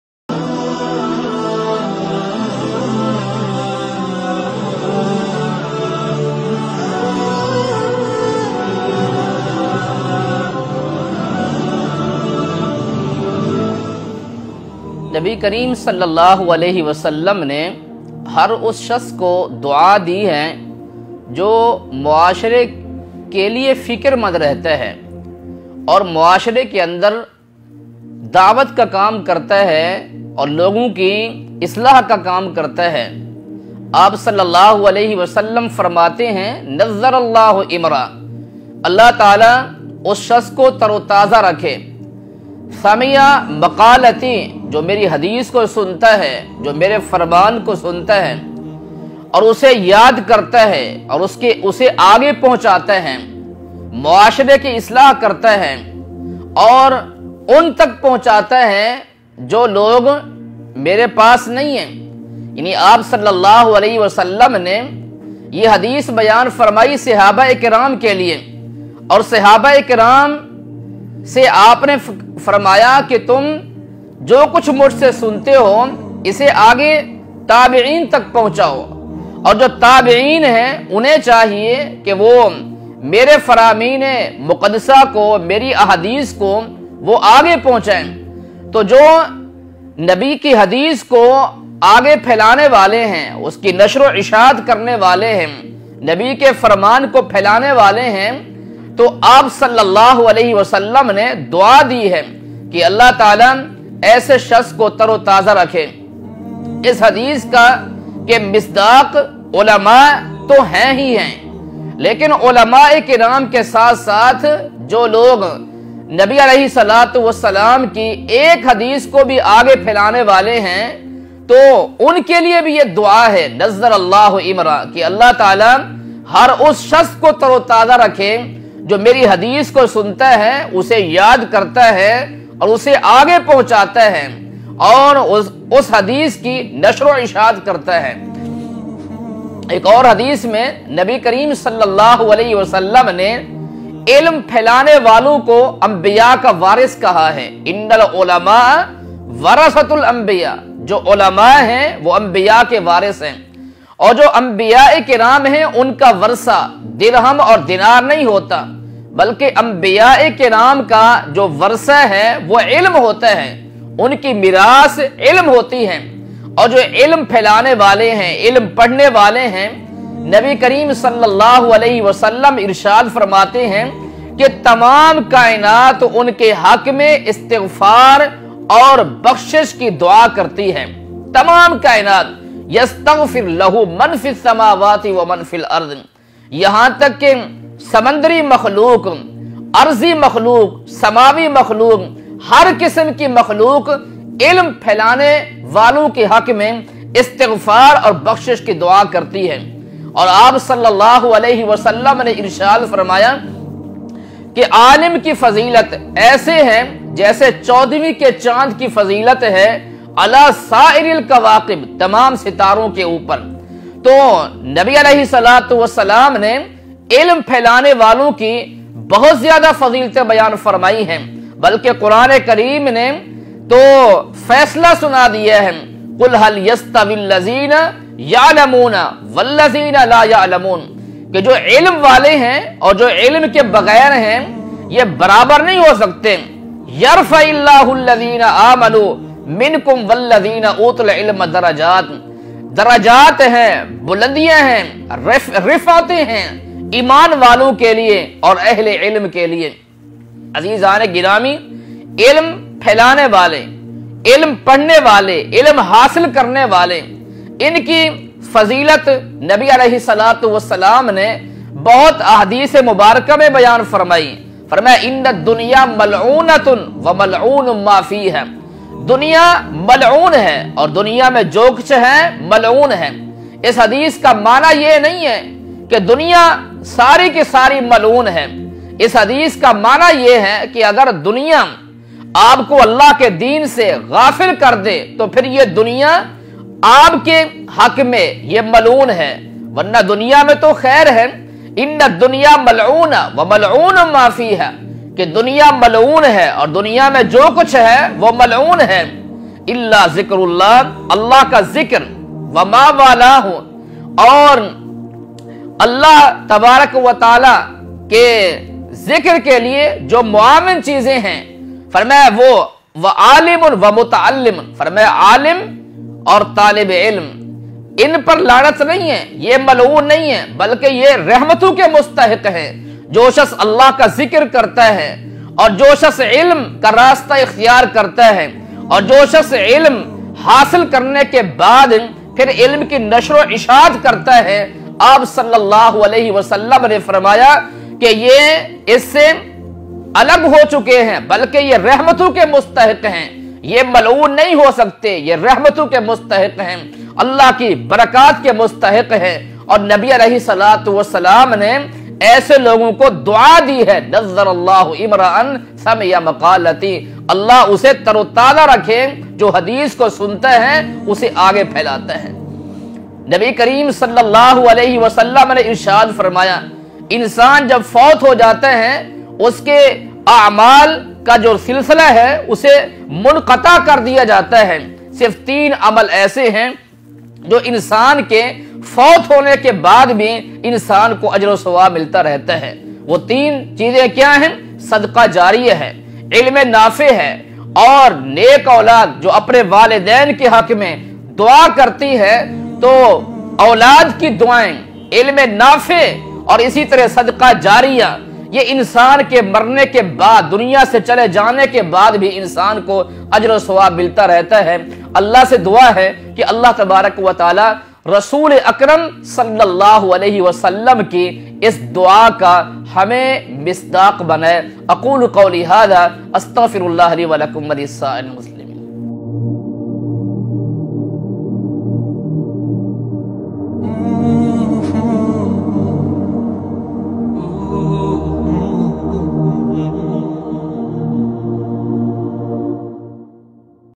Nabi(S.A.W) Ki Dua Ka Mustahiq Kon | Almidrar Institute
Weekly Reminder : Nabi(S.A.W) Ki Dua Ka Mustahiq Kon by Shaykh ul Hadeeth Khaleel ur Rahman Aziz.

Almidrar Institute
Almidrar.com
FB.com/almidrar